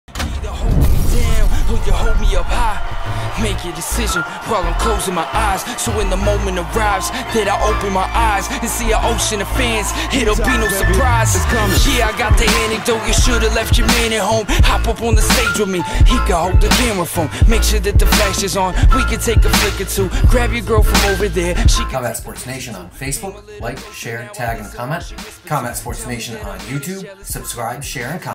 Cage Titans FC ***THUG LIFE*** Chris Taylor
There have been a lot of these short "Thug Life" videos going around, figured I would make an MMA/Fighter themed one...In the dressing room post fight hanging with the Victory Combat Academy crew. Chris Taylor left with the "W" and a cauliflower ear that looked like it was borderline going to explode!!